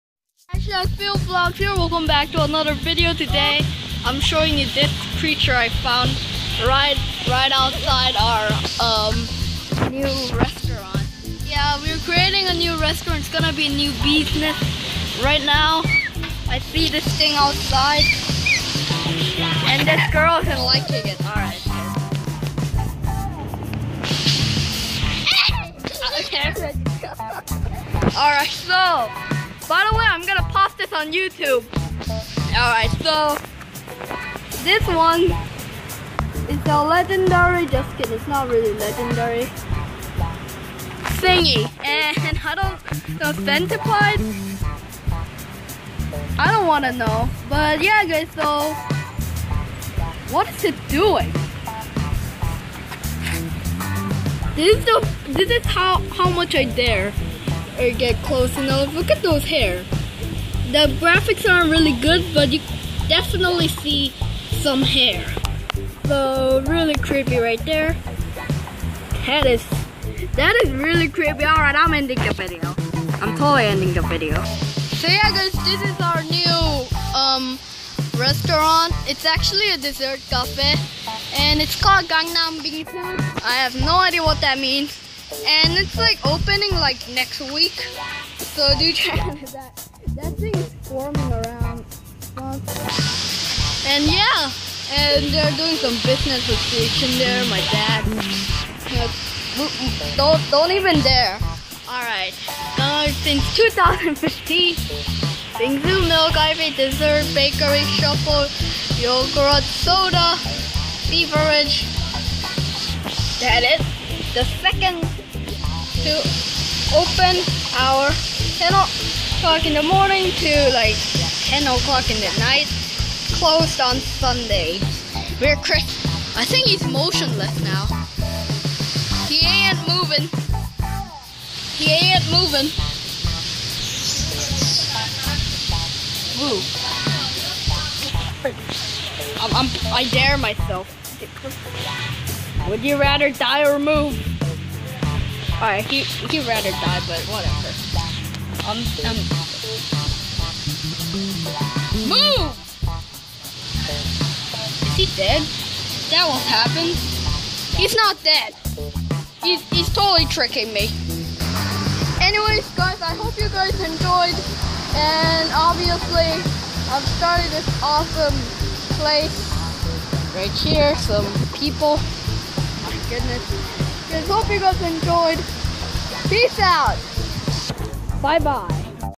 THE CENTIPIDE OF DOOM! (Hairy!)
Hello people, It is Fail_ Vlogs back with another awesome video! Today I finally reveal my parent's business! They are starting a dessert café called Gangnam Bingsu. But I have also found the CENTIPIDE OF DOOM outside!
Hey! I am in the web! Here I am!
Thx for watching, PEACE OUT!